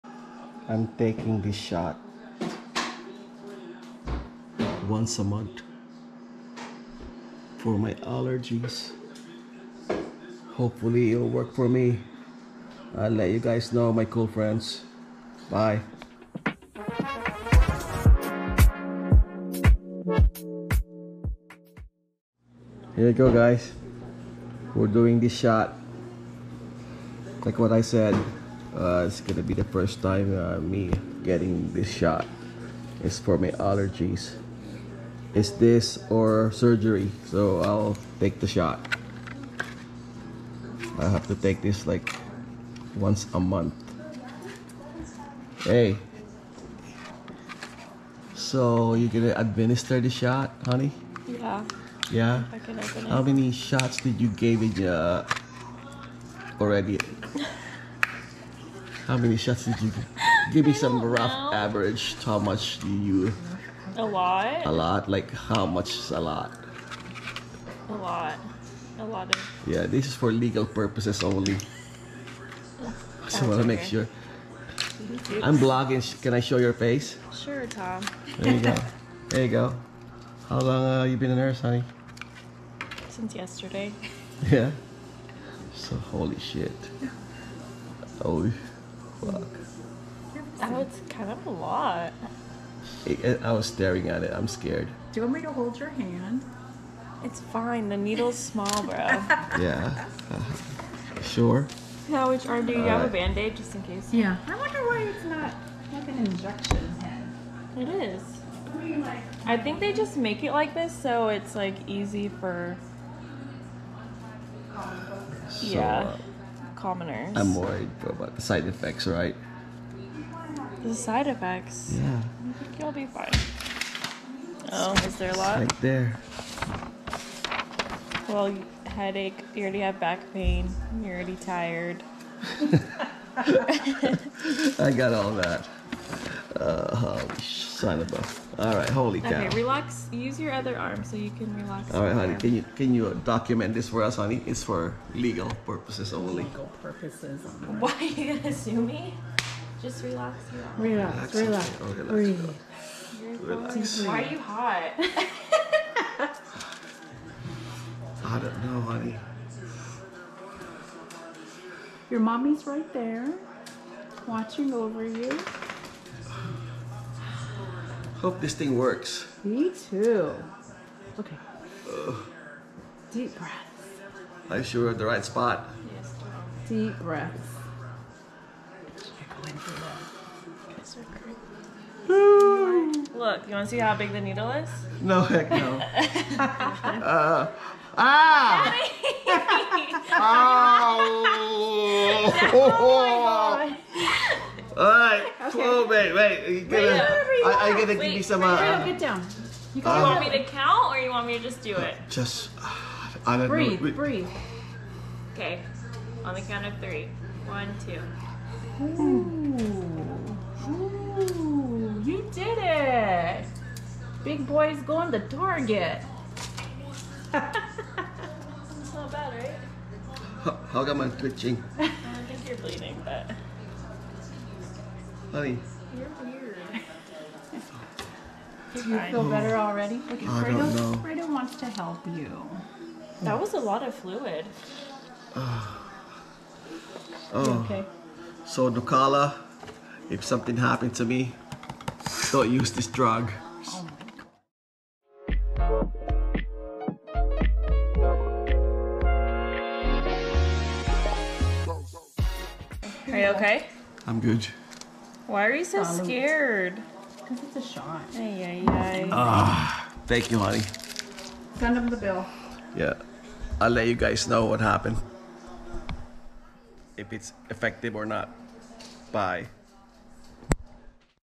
Getting My Nucala Allergy Injection - Cool Channel TV
Trying out this monthly allergy injection. Wish me luck!